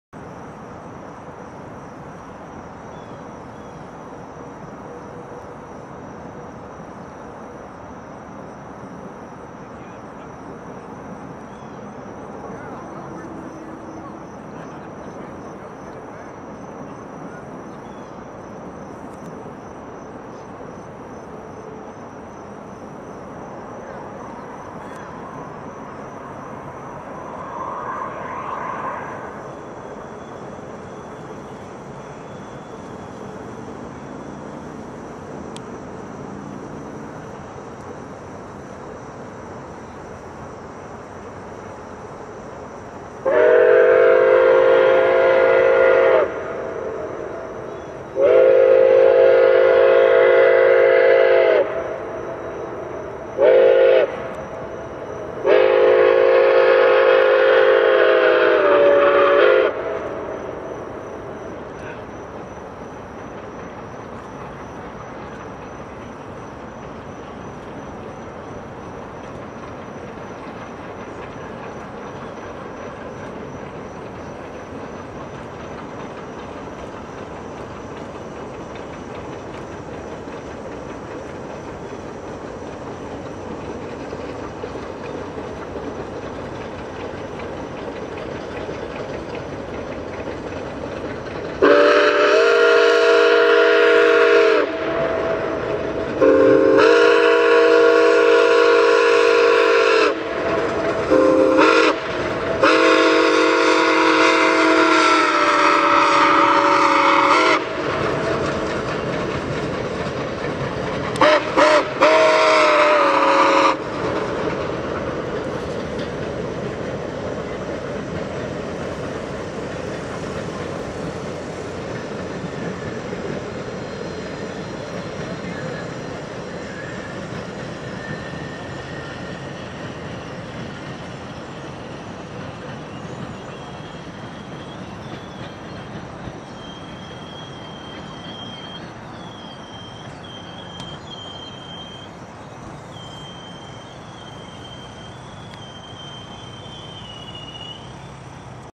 Nickel Plate engine #765 Westbound on Rockville Bridge
Nickel Plate engine #765 Westbound on Rockville Bridge, Harrisburg, PA pulling a Norfolk/Southern employee excursion trip.  Video was shot from the grounds of the Bridgeview Inn Bed & Breakfast on the west bank of the Susquehanna River on 8/19/2012.